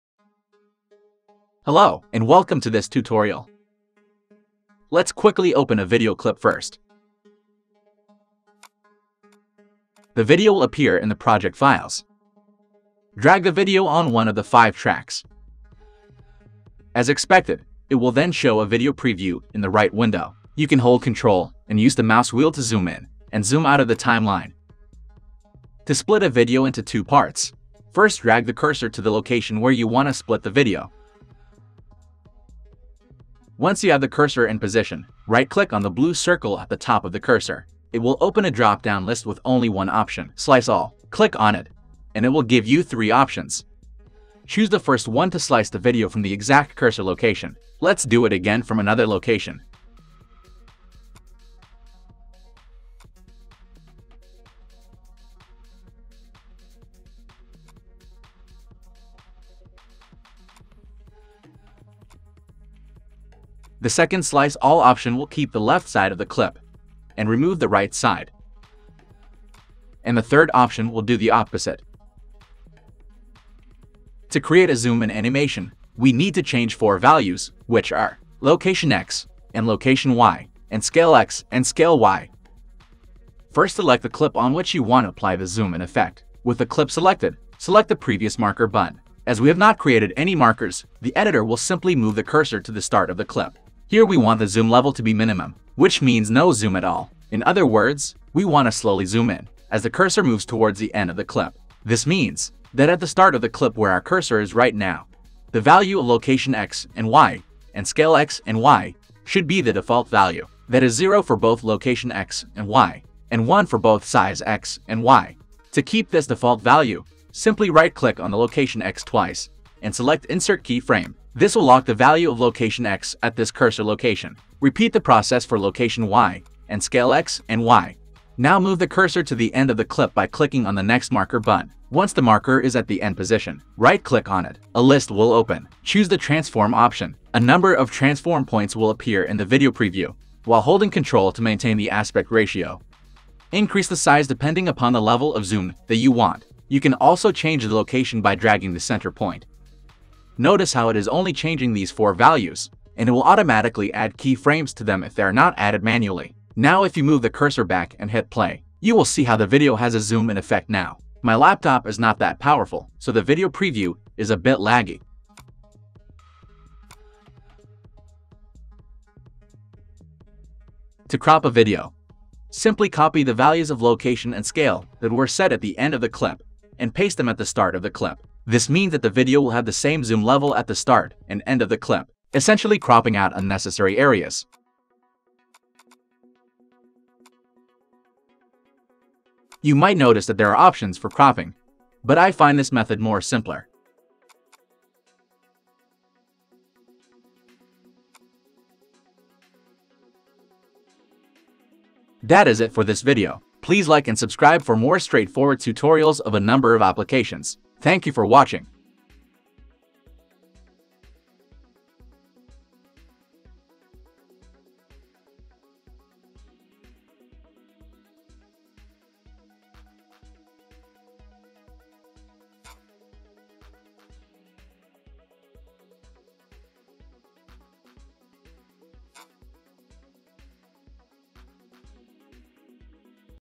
How to Cut, Split, Crop & Zoom a Video - Free OpenShot Video Editor 2021
The video will demonstrate step by step on how to Cut, Split, Crop & Zoom a Video in the free OpenShot Video Editor available for Linux, OS X, and Windows.